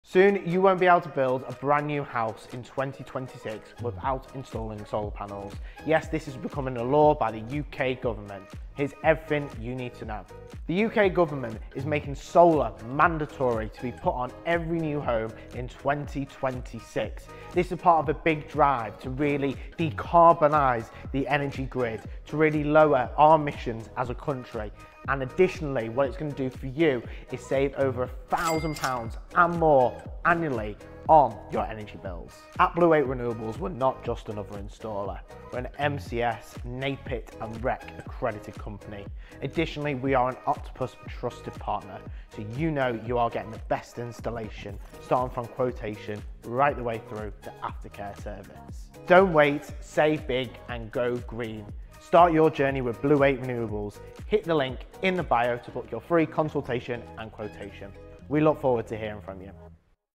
New Law: Solar Panels REQUIRED by 2026 ⚠️
The UK government has announced a major shift! From 2026, all new homes will be legally required to have solar panels installed ☀️🏠

In this short, Josh from Blue Ape Renewables breaks down what this means for future homeowners and builders.

⚡ Big changes are coming — are YOU ready?